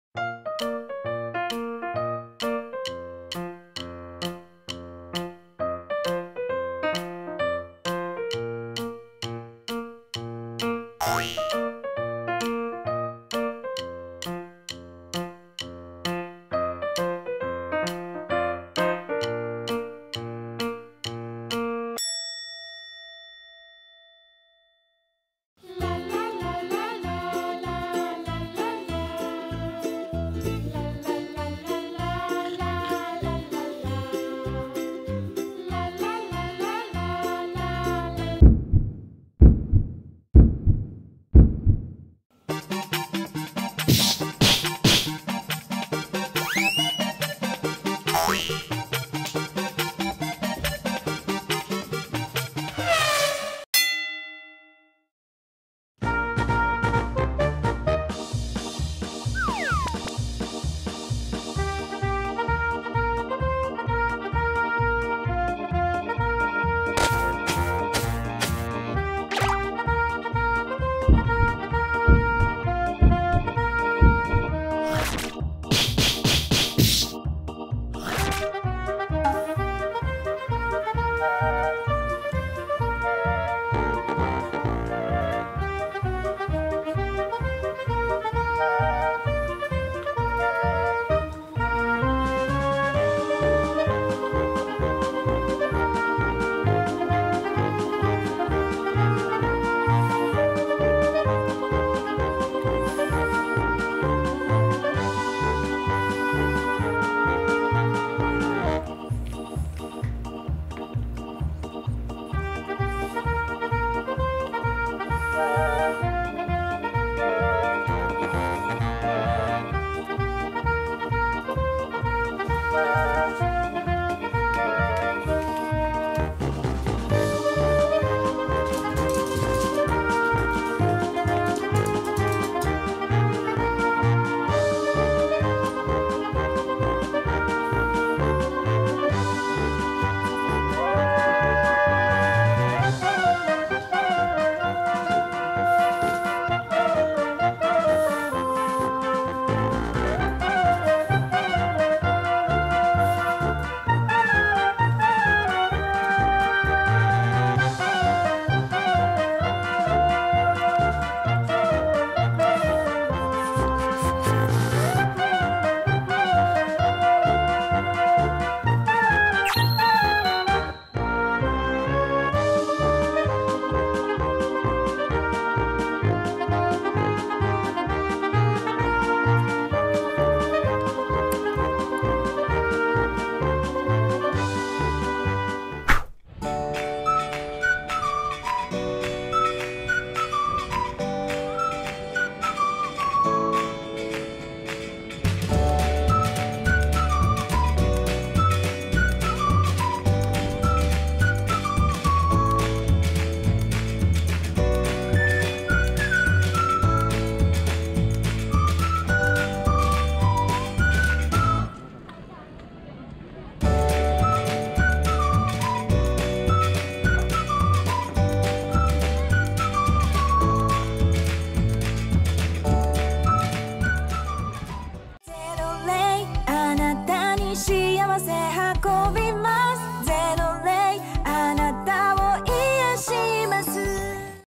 家族というよりライバル関係の犬猫コンビが面白いので見てほしいｗ
フレンチブルドッグのレイちゃんとエキゾチックショートヘアのという猫のリンちゃん。
ふたりは仲が良いのか悪いのか一見分からない関係です（笑）
やられたらやり返す関係に注目ですｗ

これからも癒しやワクワクをお届けできるように頑張りますので

よければ感想などコメントいただけると嬉しいです！
最後までありがとうございます。

ゼロ（ボストンテリア・女の子）
レイ（フレンチブルドッグ・女の子）
リン（エキゾチックショートヘア・男の子）

★ゼロレイのグッズショップ